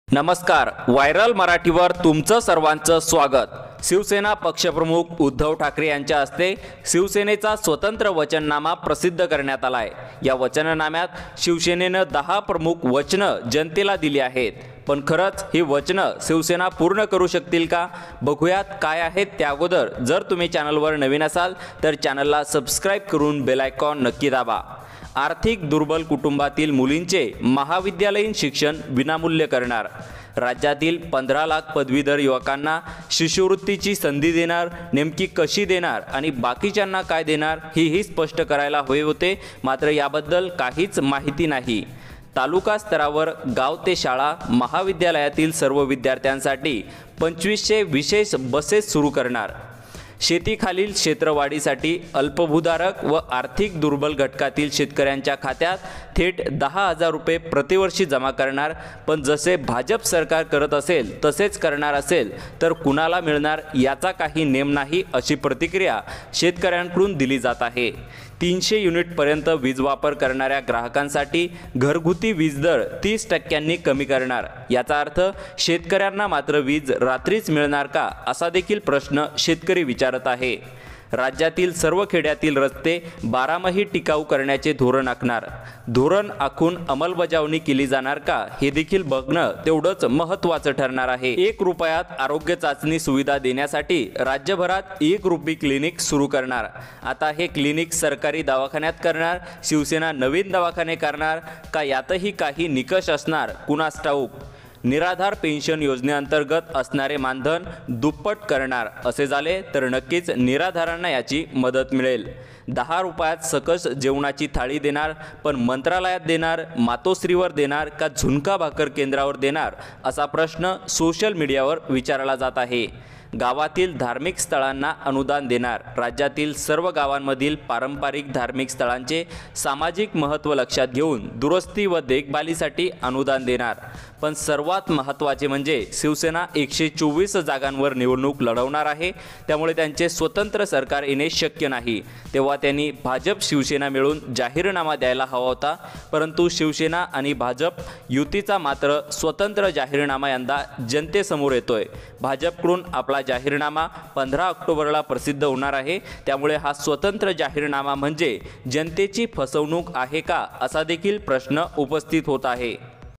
10 रुपयात जेवण कुठे मिळणार ? शिवसेना वचनणामा जाहीर Shivsena Manifesto Vidhansabha Maharashtra 2019
10 रुपयात जेवण कुठे मिळणार ? शिवसेना वचनणामा जाहीर Shivsena Manifesto Vidhansabha Maharashtra 2019

वचननाम्यातील महत्वाचे मुद्दे

प्रत्येक जिल्ह्यात 10 रुपयांत थाळी देणारे कॅन्टीन उभारणार

1000 भोजनालयं बनवणार त्यात 10 रुपयांत उत्तम भोजन

शेतकऱ्यांसाठी दरवर्षी दहा हजार रूपयांची योजना

एका रुपयात 200 पेक्षा अधिक आजारांवर आरोग्य चाचणी

आर्थिक दुर्बल कुटुंबातील मुलींचे महाविद्यालयीन शिक्षण विनामूल्य करणार

मुख्यमंत्री ग्रामसडक योजनेच्या धर्तीवर मुख्यमंत्री शहर सडक योजना

15 लाख पदवीधर युवकांना 'युवा सरकार फेले अंतर्गत शिष्यवृत्तीची संधी देणार

शेतकऱ्यांचा सात बारा कोरा करणार

प्रत्येक जिल्ह्यात वैद्यकीय महाविद्यालयासह सुपर स्पेशालिटी हॉस्पिटल उभारणार

ज्येष्ठ नागरिक आणि महिलांसाठी अनेक योजना

घरगुती वीजदर कमी 30टक्क्यांपर्यंत कमी करणार

शेतकऱ्यांना कर्जमुक्त करणार

ग्रामीण विद्यार्थ्यांसाठी विशेष बसेसची सोय

तीर्थक्षेत्रांच्या प्रवासासाठी विशेष समन्वय केंद्रांची स्थापना

पीकविमा योजनेत नुकसानग्रस्त शेतकऱ्यांना मदत मिळण्याची सोय करणार

पाच वर्षासाठी खतांचे भाव स्थिर ठेवणार

The Shiv Sena on Saturday promised cheap meals, lower electricity tariffs, cash transfers to farmers and scholarships to students among other sops in its manifesto for the October 21 assembly elections in Maharashtra.

Shiv Sena chief Uddhav Thackeray released the party’s manifesto for the Maharashtra assembly elections 2019 at his residence ‘Matoshree’ in Bandra. He was accompanied by his son and Sena leader Aaditya Thackeray.

“There is no doubt about the Shiv Sena’s coalition with the BJP. The manifesto is the Sena’s promises. We also tried to keep in mind how the state treasury will be affected by our decisions and promises,” Uddhav Thackeray said.

The Shiv Sena promised to provide wholesome meals across the state at Rs 10, health tests at Re 1, reduce electricity charges up to 300 units by 30%, direct cash transfers of Rs 10,000 every year for farmers from weaker sections, scholarships to 15 lakh youth.